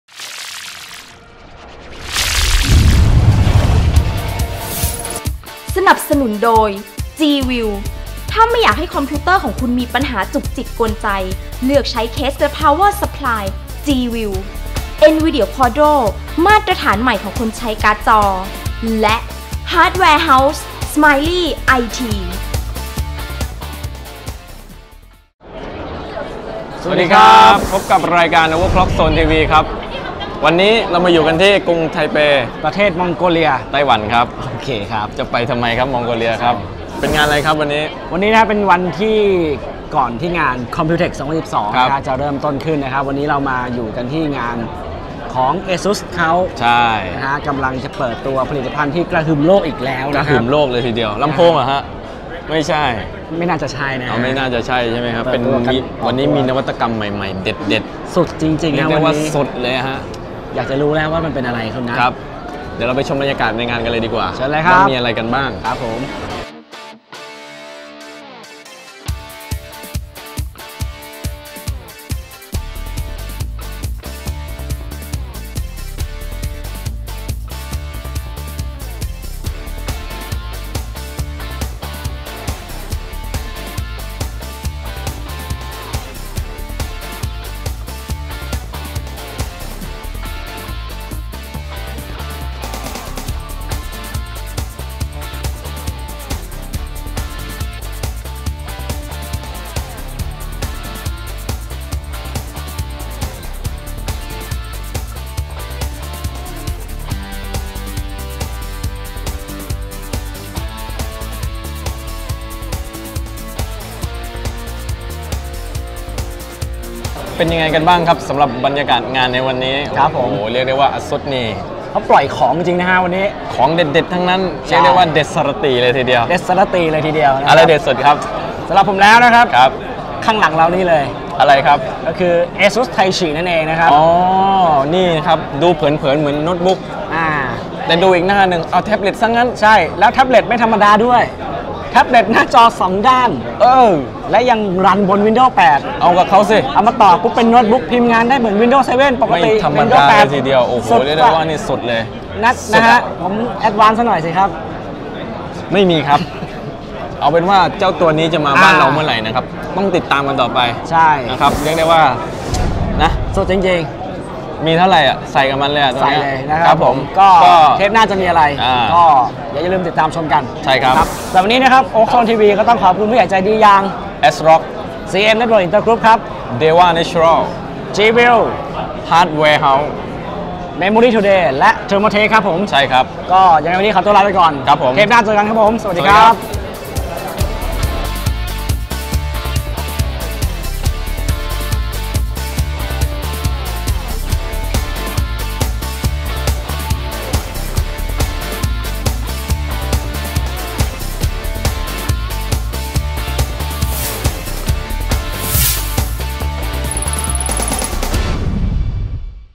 COMPUTEX TAIPEI 2012 Report: ASUS TAICHI Launching!
เทปแรกกับการรายงานสด COMPUTEX TAIPEI 2012 จากกรุงไทเป ประเทศไต้หวัน
พบกับ ASUS TAICHI นวัตกรรมล่าสุดจาก ASUS แล๊ปท๊อปลูกผสมแท๊ปเลต
ที่มาพร้อมหน้าจอแบบ Dual-Screen บนระบบปฏิบัติการ Windows 8!!
ที่สุดแห่งนิยาม Incredible Innovation, Endless Possibilities!